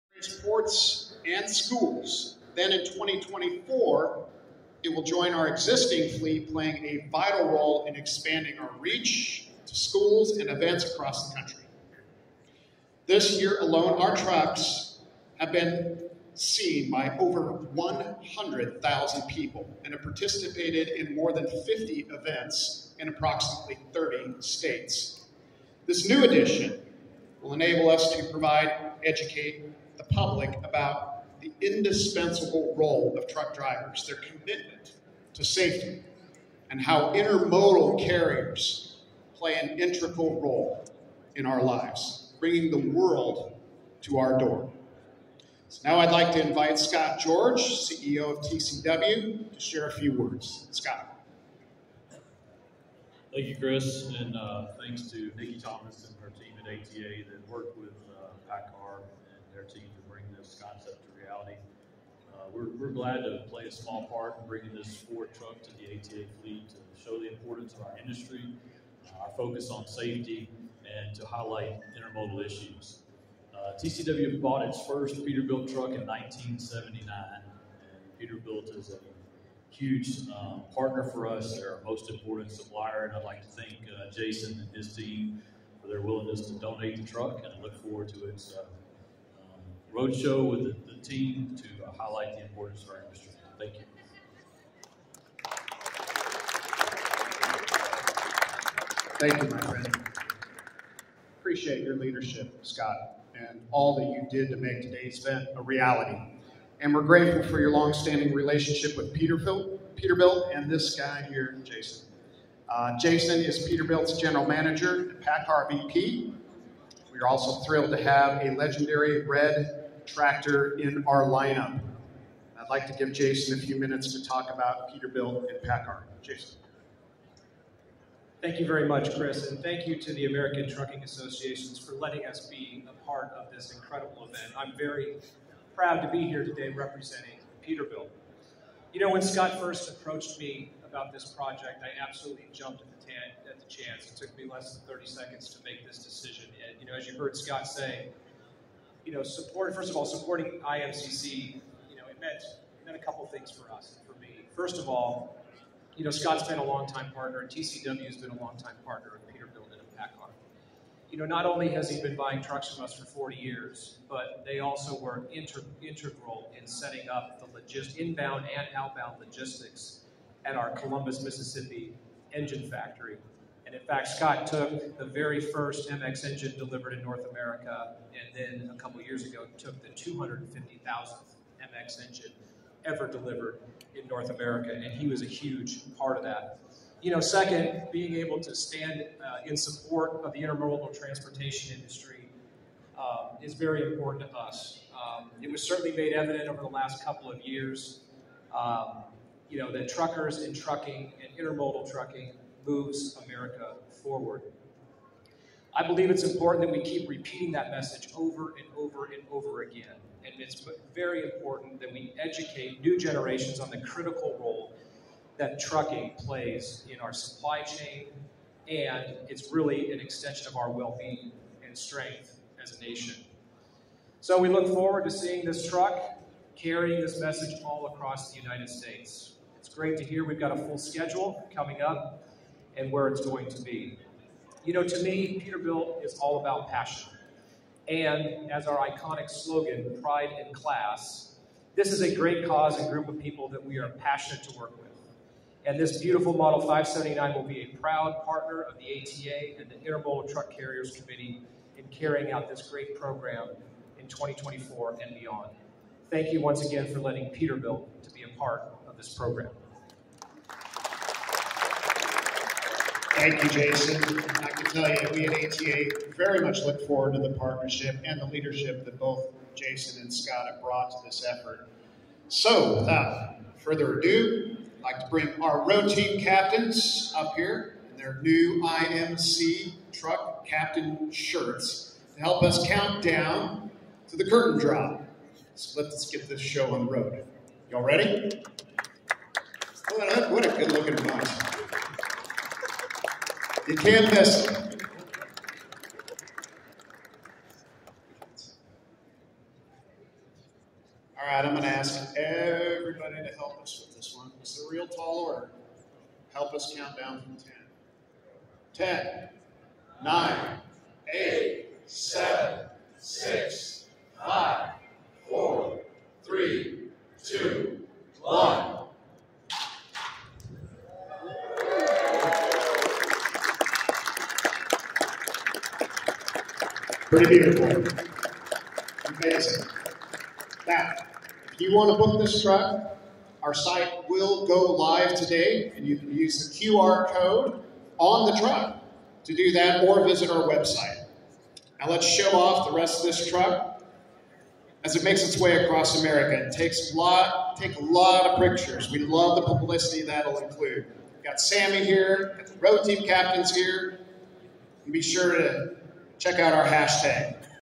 American Trucking Associations drops the curtain on its new Intermodal Peterbilt truck
The truck, unveiled at ATA's MC&E conference, will travel the country to bring awareness to the importance of trucking and truck drivers.